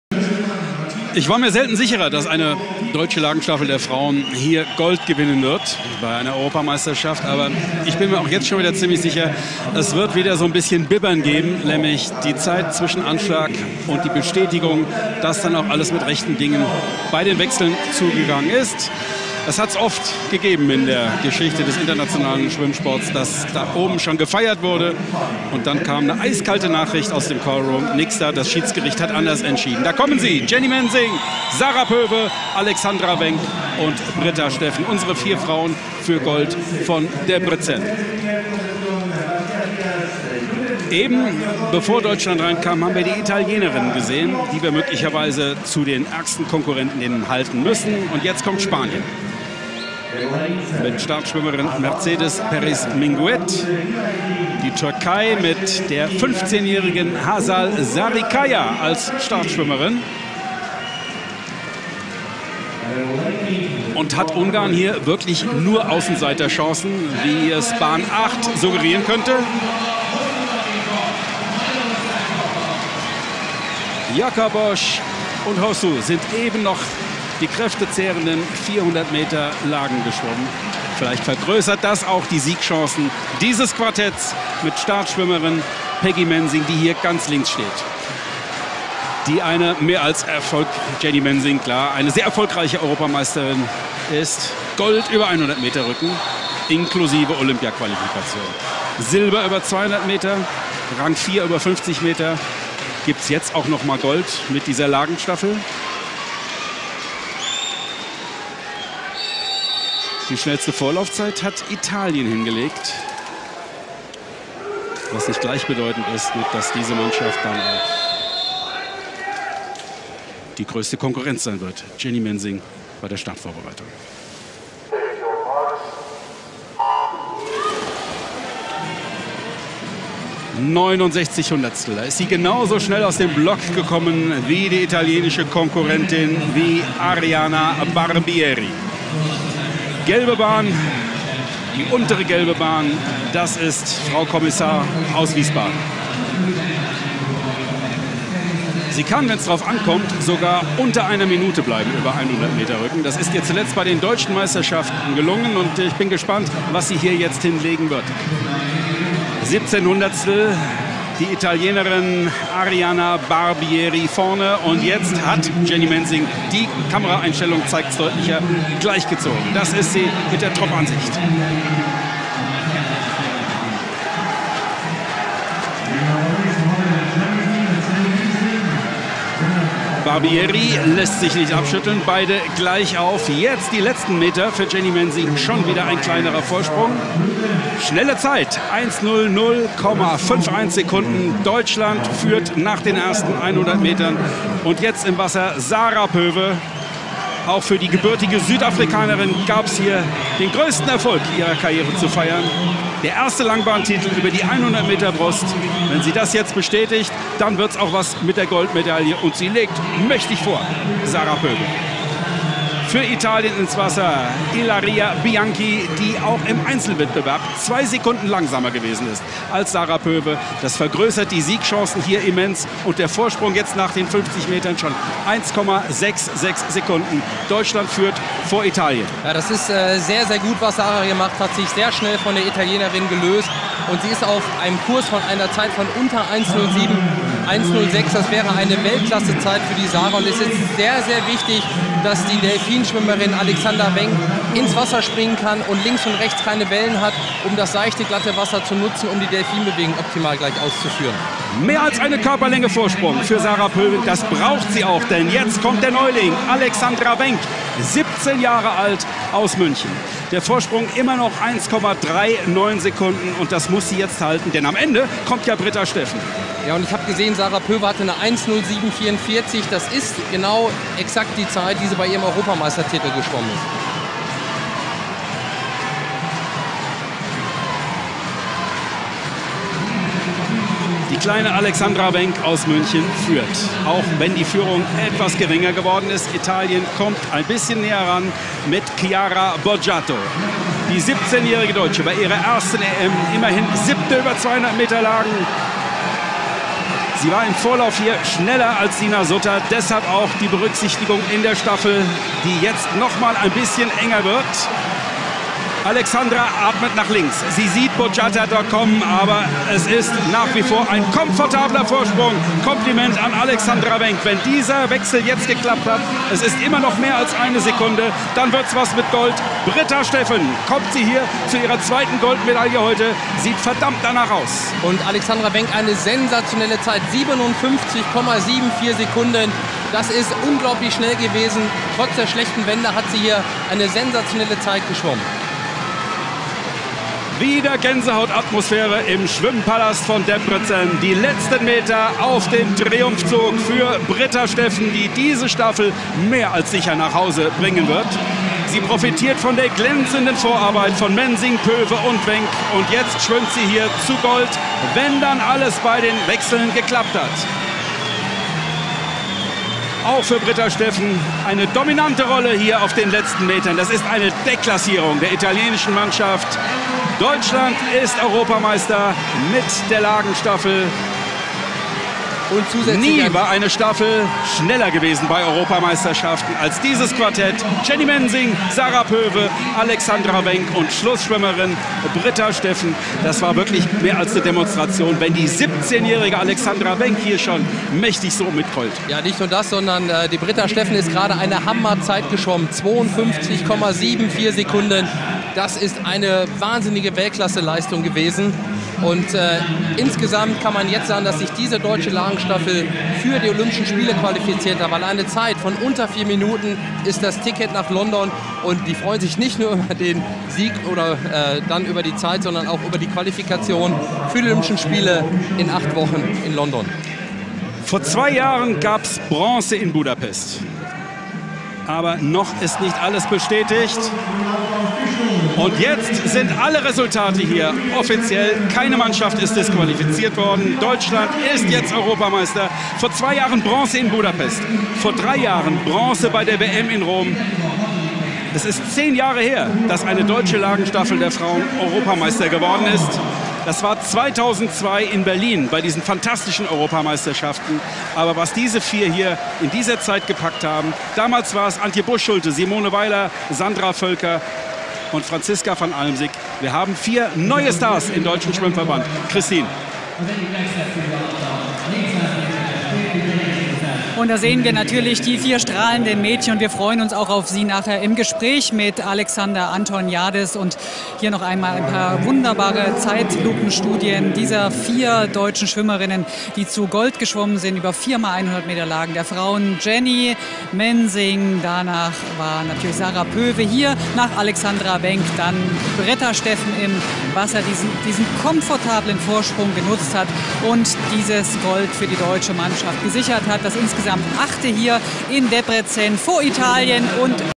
Женская Сборная Германия устанавливает Новый Рекорд Чемпионатов Европы!
Чемпионат Европы по плаванию на длинной воде в Дебрецене 2012
Женщины - 4х100 м Комбинированная эстафета
1. Сборная Германии - 3.58.43
2. Сборная Италии - 4.01.92
3. Сборная Швеции - 4.05.58
4. Сборная Исландии - 4.06.64
5. Сборная Испании - 4.06.78
6. Сборная Венгрии - 4.07.19
7. Сборная Финляндии - 4.07.55
8. Сборная Турции - 4.07.56